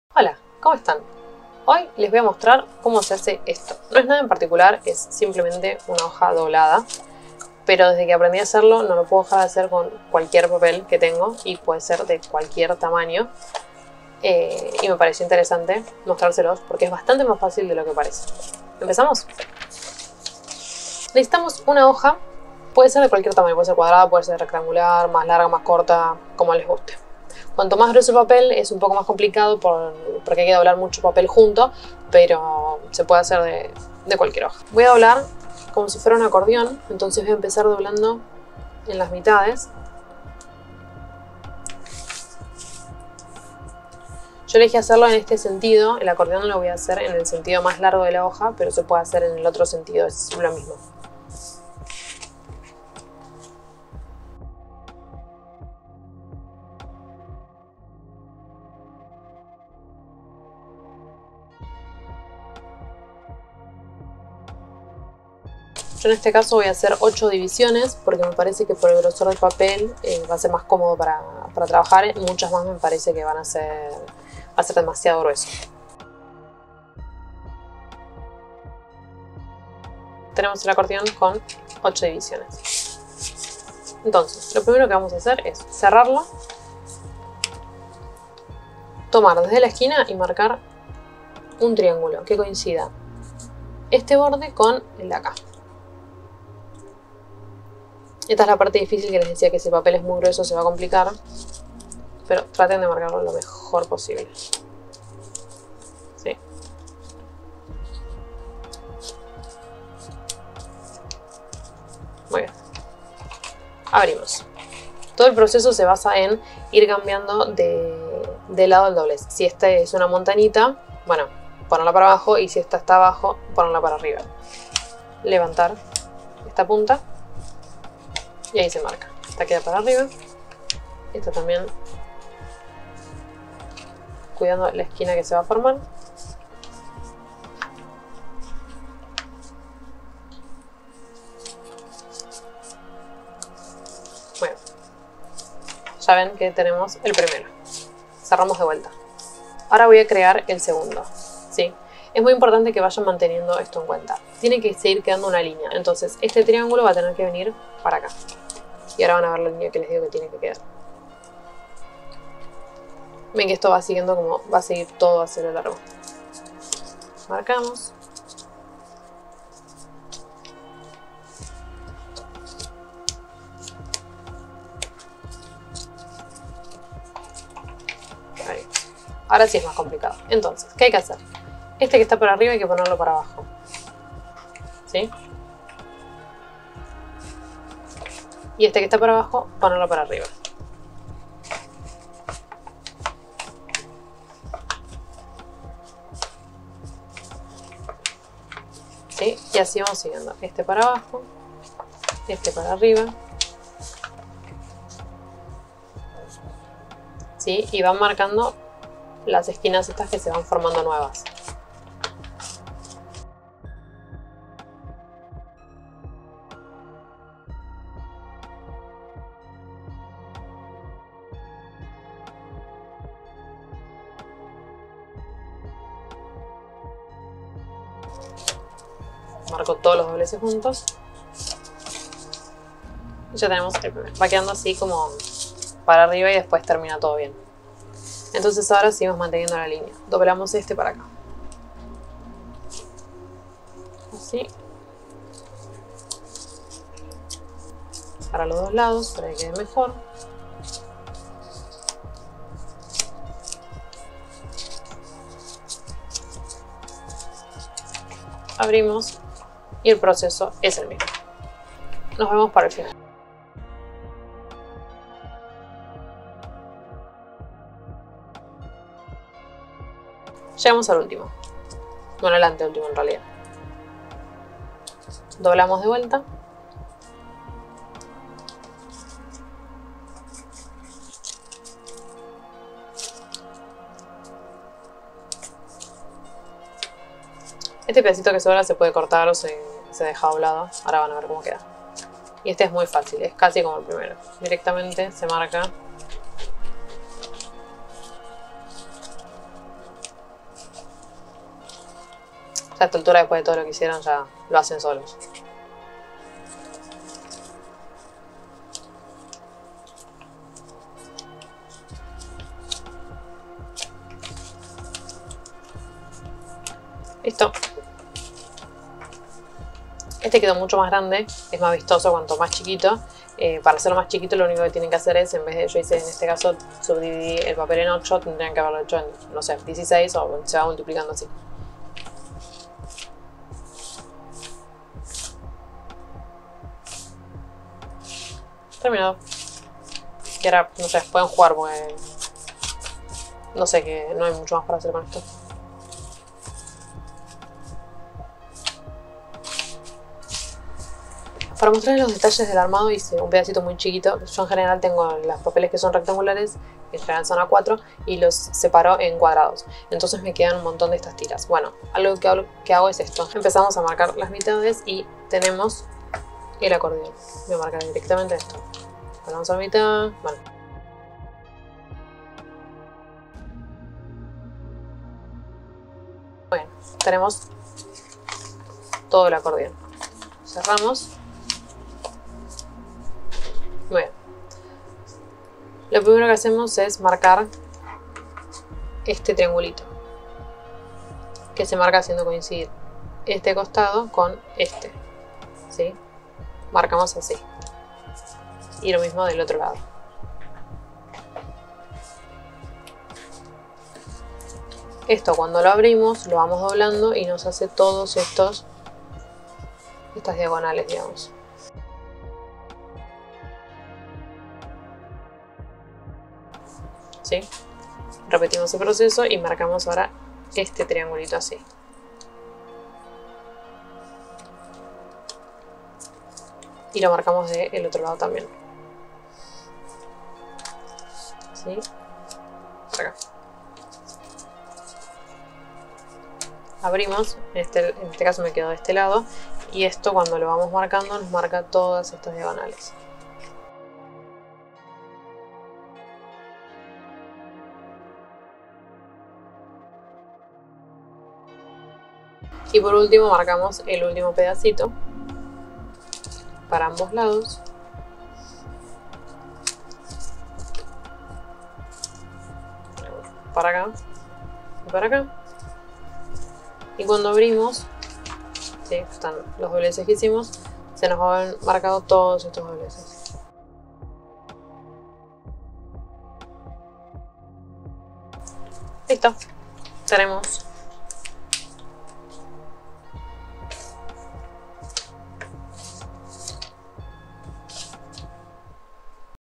Patrón zig zag origami FACIL | origami herringbone tessellation
Se llama "origami herringbone tessellation" le decimos "papel doblado en zig zag" 😉
Contame que te pareció

Nos vemos en el próximo

Hice un calendario 2021 en formato A3 y A4 listos para imprimir!